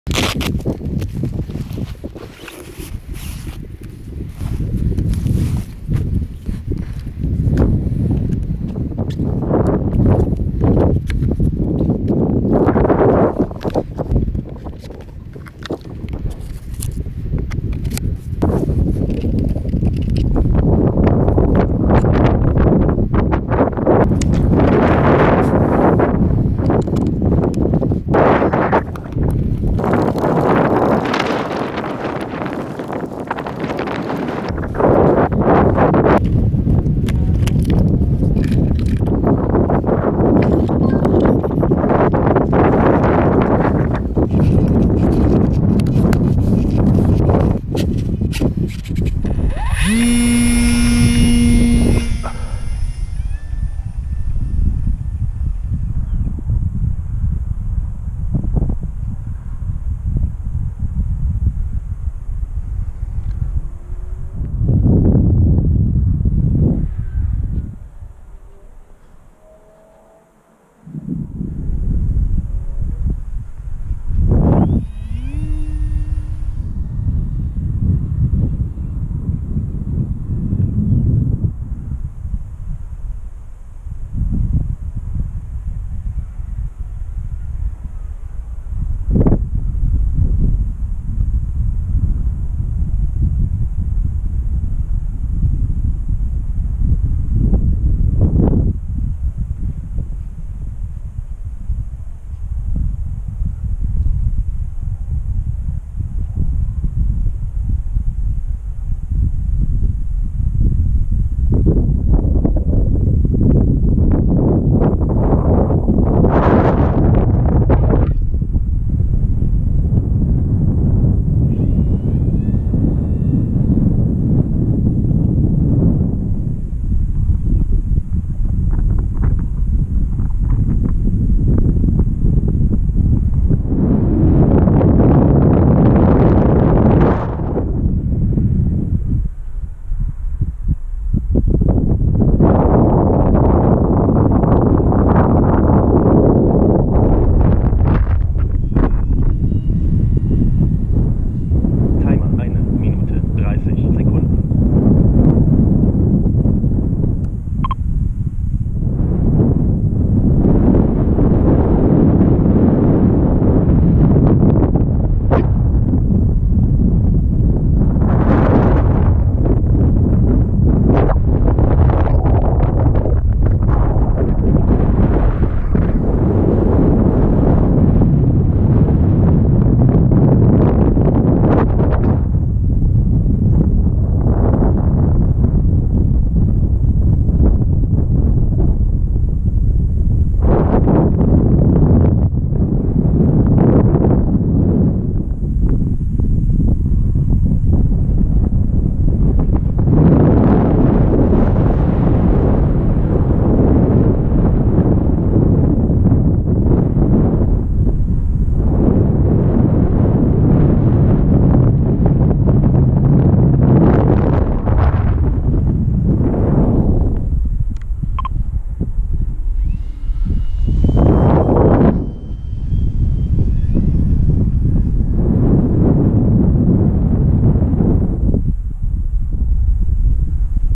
Sparrow V2.1 Testflug neues CFK Rumpfrohr
Noch schnell bevor die Sonne untergeht den neuen Rumpf testen. Aus Zeitgründen nur auf der Wiese um die Ecke, da wars sehr böig, viel Wind. Aber das Handling und die Festigkeit passt, so kann der Flieger erstmal bleiben. Das selbstgebaute CFK-Rumpfrohr ist sehr leicht aber auf jeden Fall fest genug für meine Ansprüche. Insgesamt ist der Flieger in der aktuellen Version sehr robust und "alltagstauglich" durch die teilbare Fläche und die abnehmbaren Leitwerke. Ca. 190€ Gesamtkosten (ohne Empfänger und Sender) sind auch erträglich...